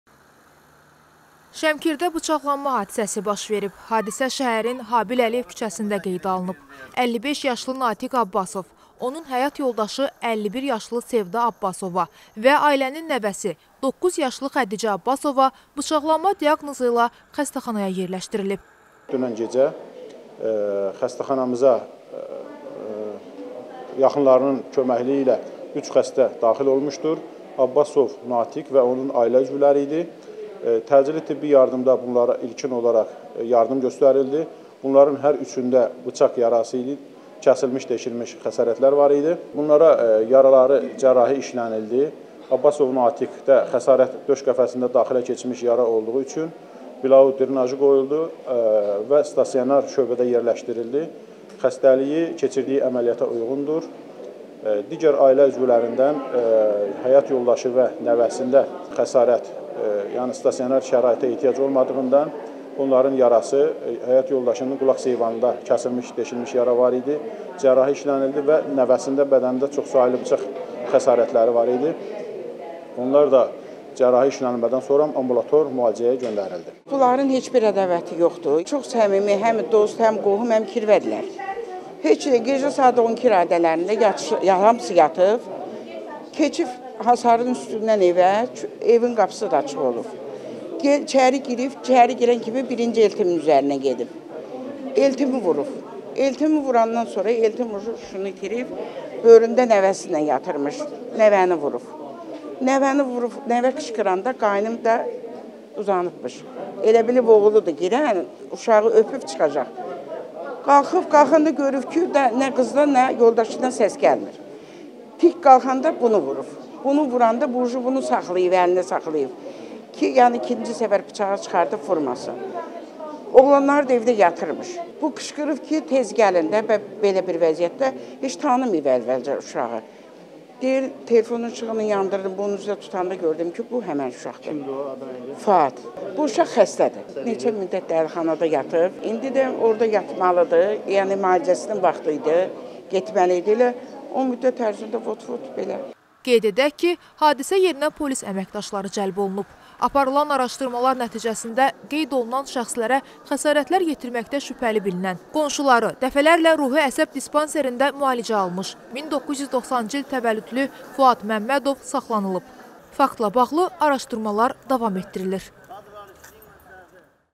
Ruhi xəstə baba, nənə və nəvəni bıçaqladı - BAKU TV
Şəmkirdə bıçaqlanma hadisəsi baş verib. Hadisə şəhərin Habil Əliyev küçəsində qeydə alınıb.
55 yaşlı Natiq Abbasov, onun həyat yoldaşı 51 yaşlı Sevda Abbasova və ailənin nəvəsi 9 yaşlı Xədicə Abbasova bıçaqlanma diaqnozu ilə xəstəxanaya yerləşdirilib. Onlardan iki nəfərə (qadın) zəruri tibbi xidmət göstərilib, ambulator müalicə üçün evə buraxılıblar. Bir nəfərin (kişi) döş qəfəsinin arxa divarının açıq yarası diaqnozu ilə müalicəsi tibb müəssisəsində davam etdirilir. Vəziyyəti stabildir.
Qeyd edək ki, hadisə yerinə polis əməkdaşları cəlb olunub. Aparılan araşdırmalar nəticəsində qeyd olunan şəxslərə xəsarətlər yetirməkdə şübhəli bilinən qonşuları, dəfələrlə ruhi-əsəb dispanserində müalicə almış 1990-cı il təvəllüdlü Fuad Məmmədov saxlanılıb. Faktla bağlı araşdırmalar davam etdirilir.

Ruhi xəstə baba, nənə və nəvəni bıçaqladı